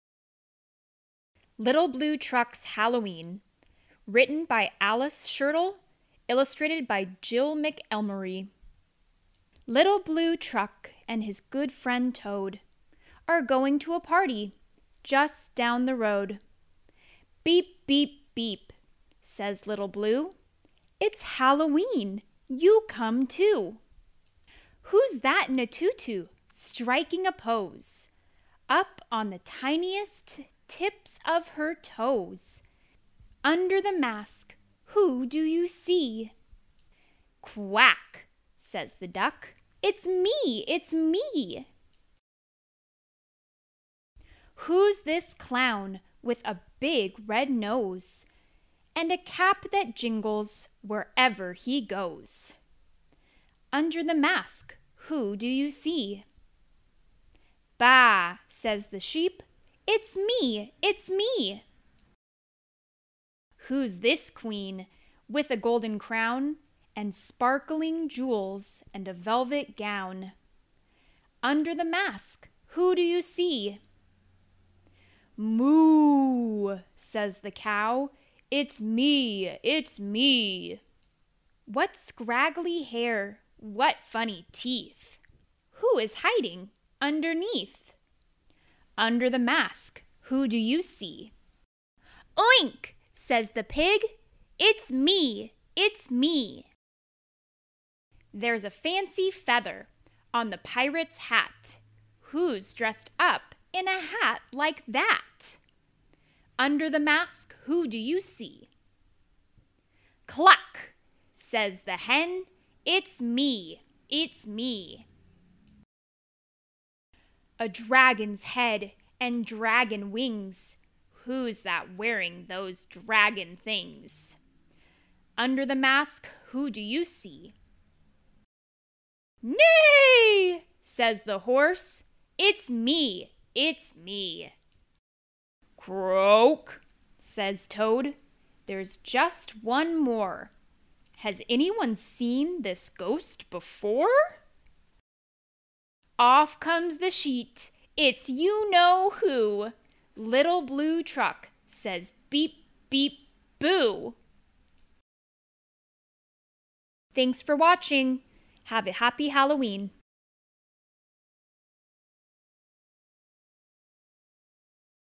Little Blue Truck's Halloween - A Read Out Loud Animation
Come join Little Blue Truck and all his friends as they celebrate Halloween in this adorable book by Alice Schertle.  This series is a favorite in our classrooms and this Halloween edition is an extra special October treat! Kids will love the illustrations by Jill McElmurry, and adults will find the book a fun listen or read!  Happy Halloween!

As always, please purchase a physical copy to support the author and read along with us at home or in the classroom.